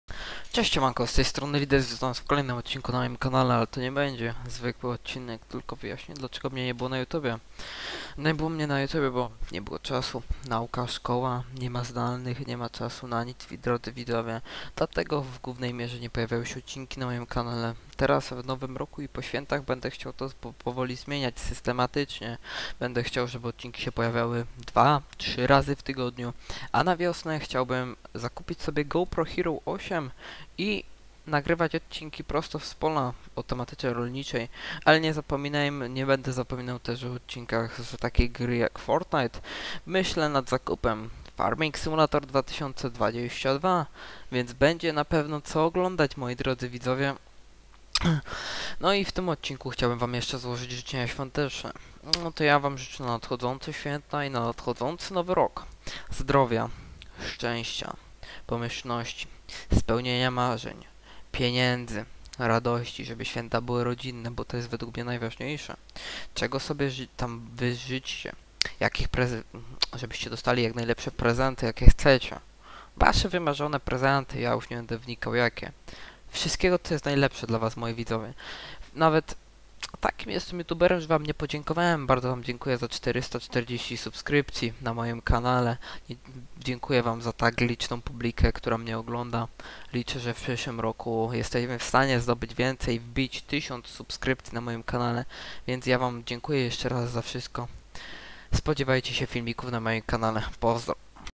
Wesołych Świąt!!!!
Siema w tym odcinku chce wam złożyć najserdeczniejsze życzenia.